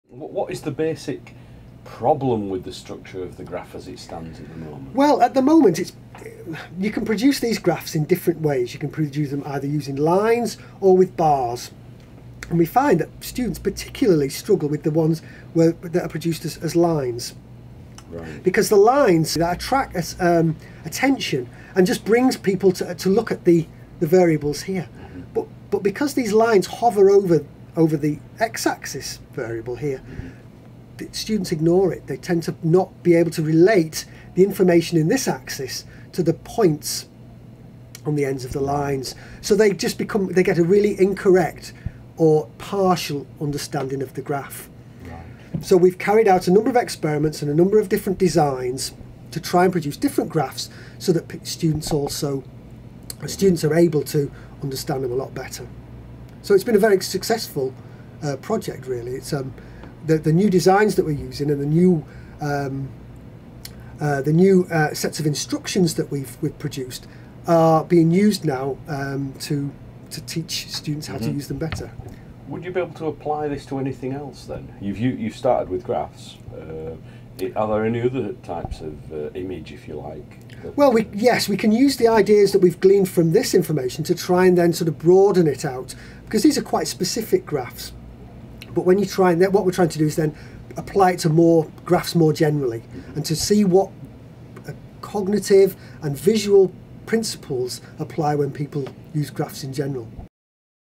Dr David Peebles talks about his eye-tracking research
David Peebles, a cognitive psychologist lecturer at the University of Huddersfield, talks about his work on why psychology students struggle to understand line graphs.